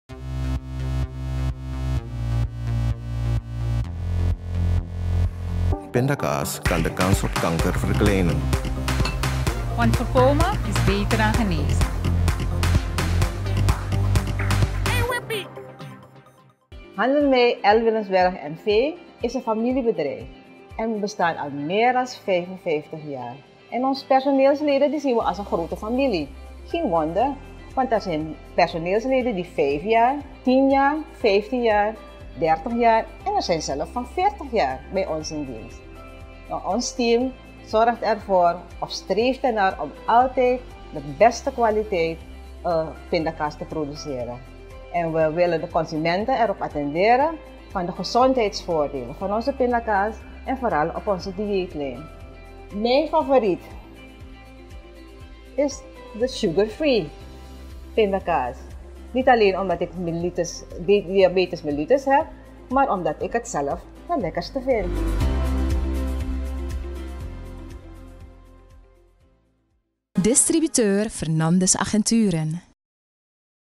Wippy Pindakaas! Al langer dan 35 jaar.
Dit is het slot van de infomercial van Wippy Pindakaas. De Algemene Directeur geeft aan welke variant haar favoriet is en waarom.